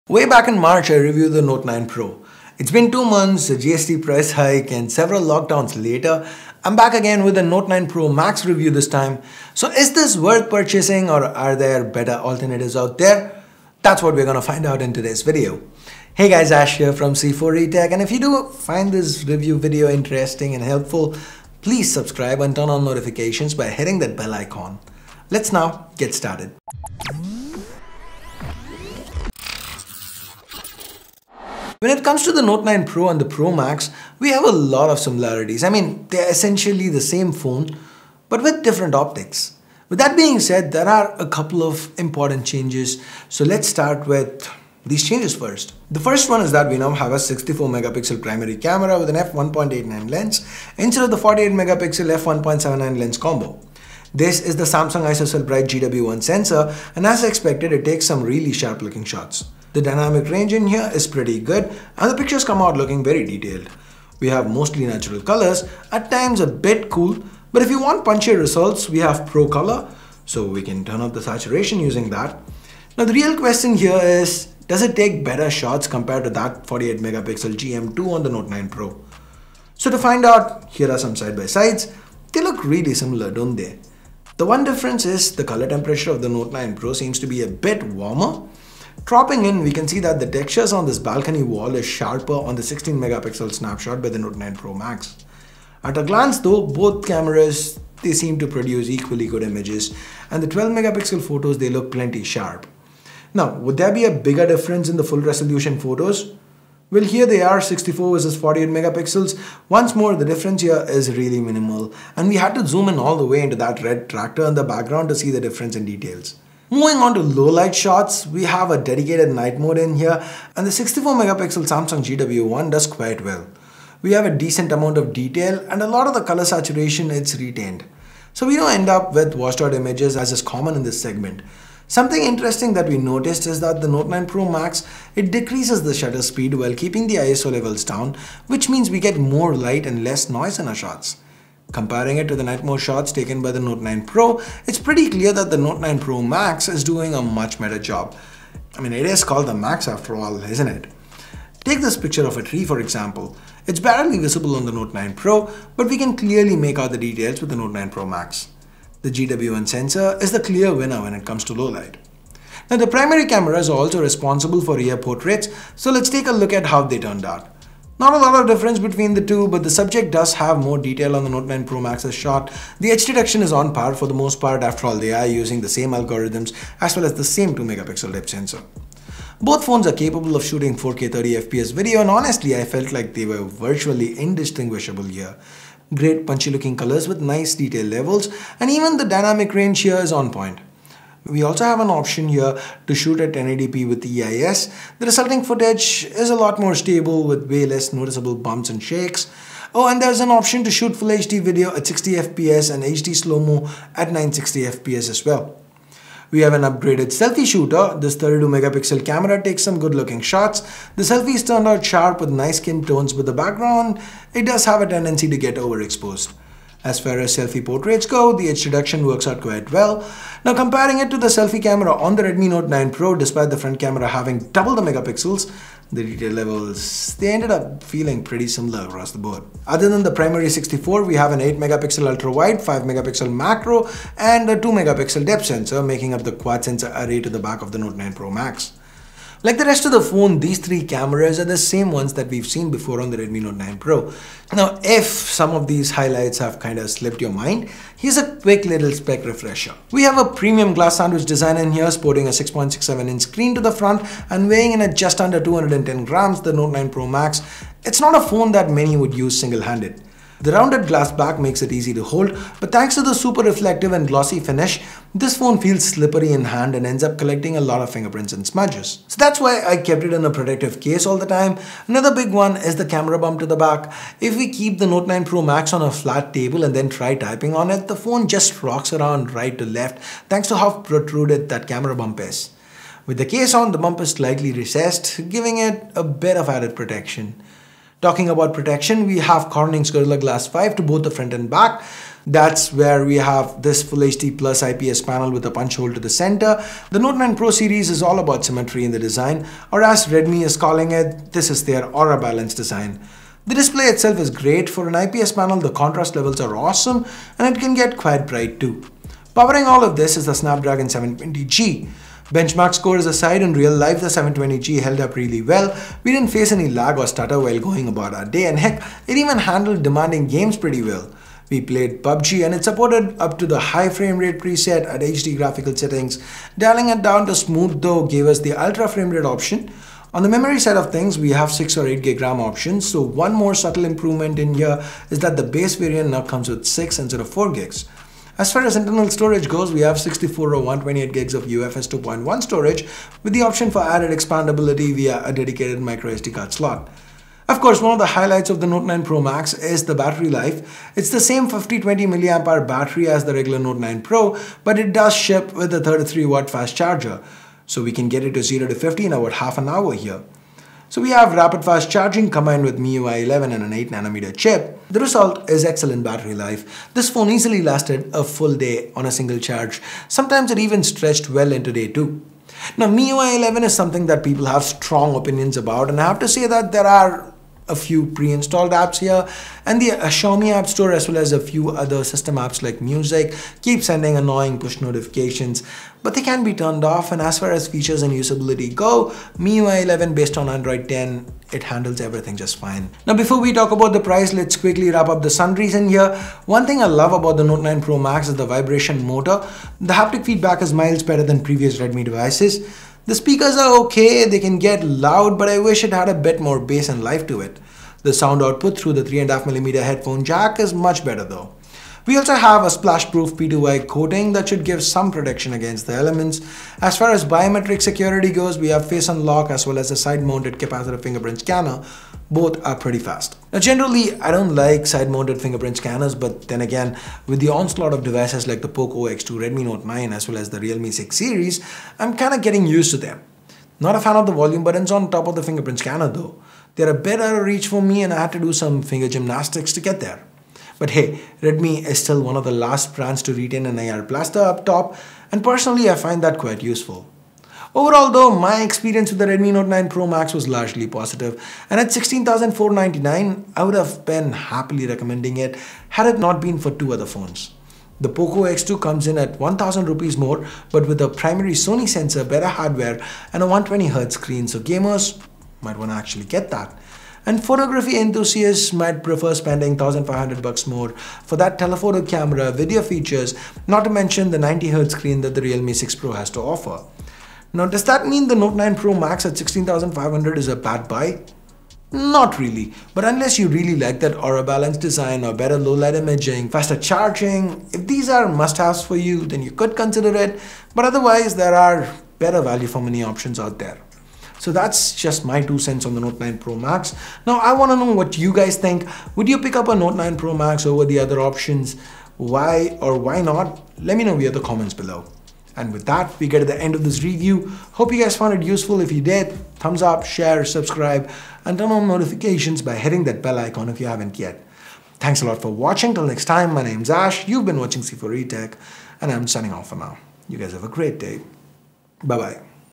Redmi Note 9 Pro Max Review - There’s Better Out There!!!
In this video we review the Redmi Note 9 Pro Max and tell you guys if you should spend your hard earned money on it!

The Redmi Note 9 Pro Max is basically the same phone as the Redmi Note 9 Pro with an improved rear primary sensor that is now 64MP, an upgraded selfie camera that’s now 32MP and faster 33 Watt charging support!